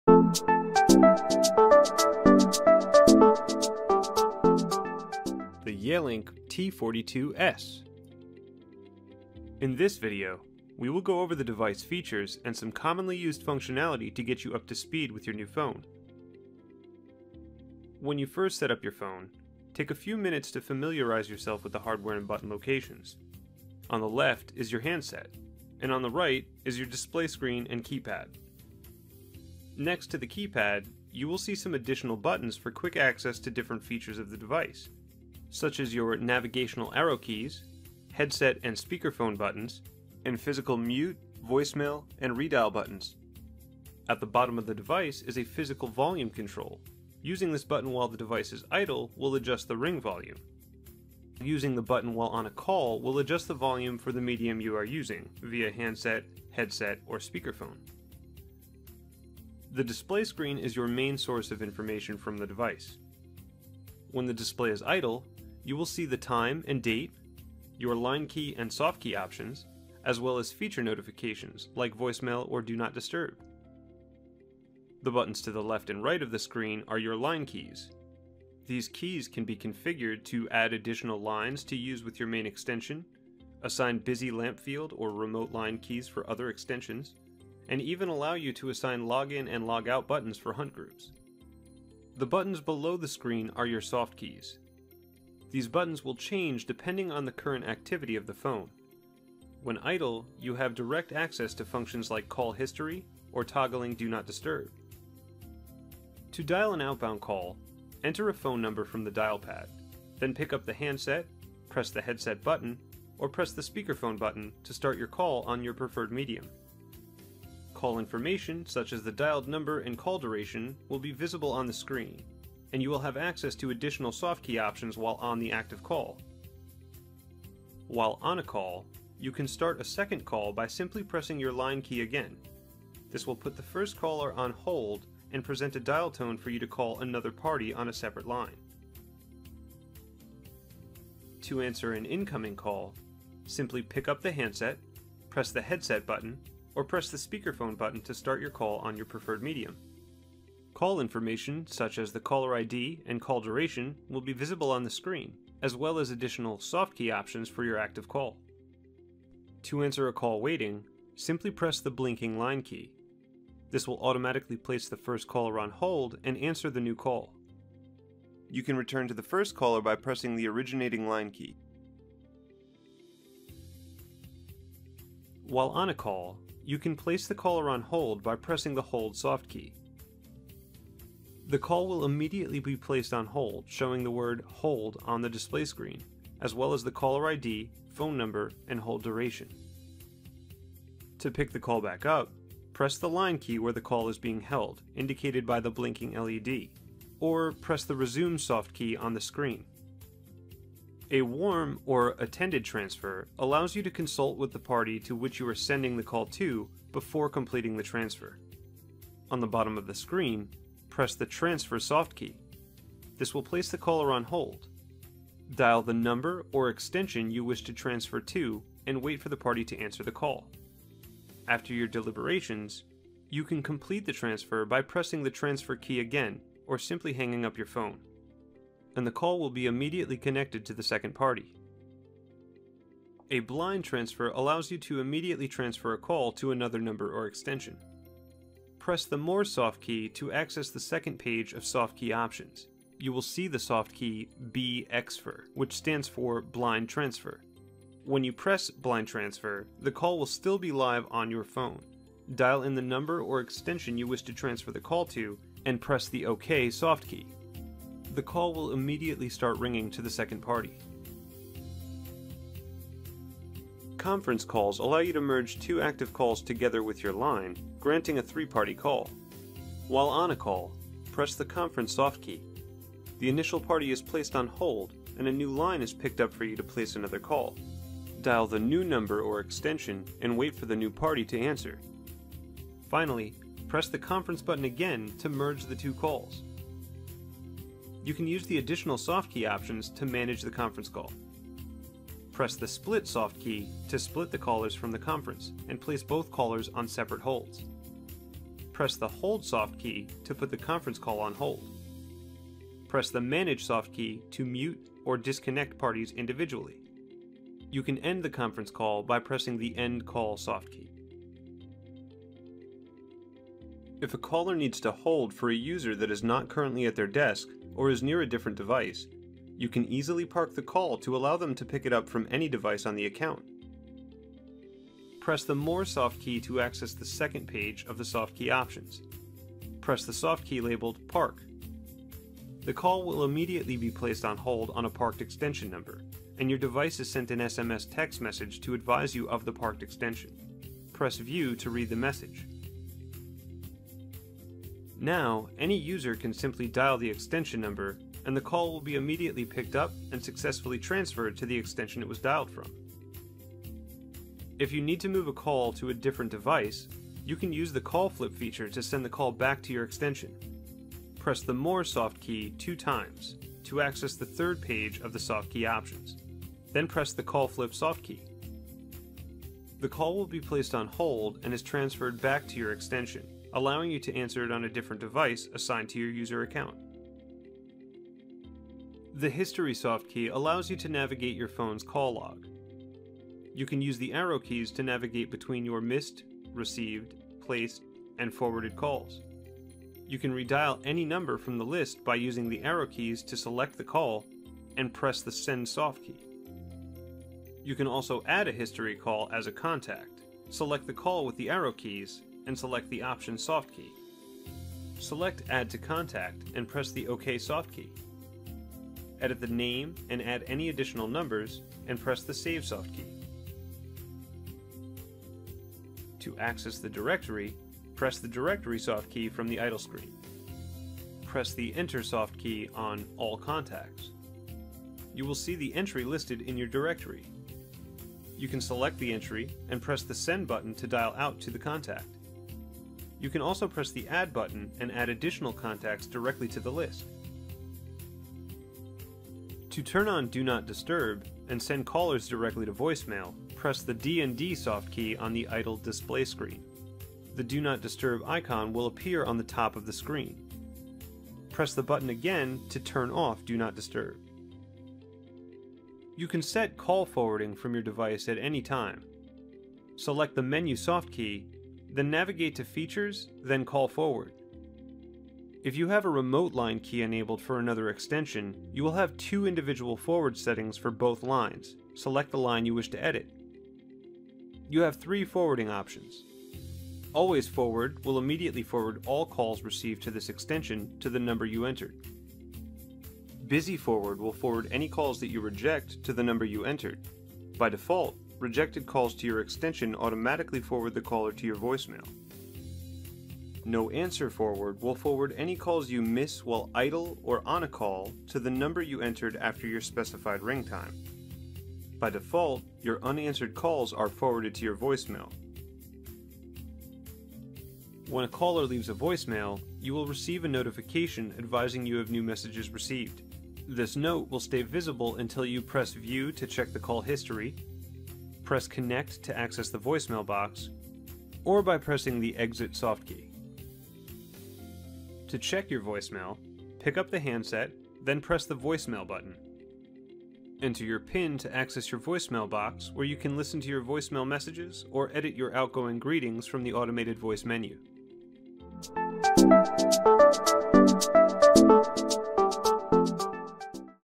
Yealink T42 - How To Guide
This video will walk you through the features and functionality, and how to use your YealinkT42S Call Conferencing Device.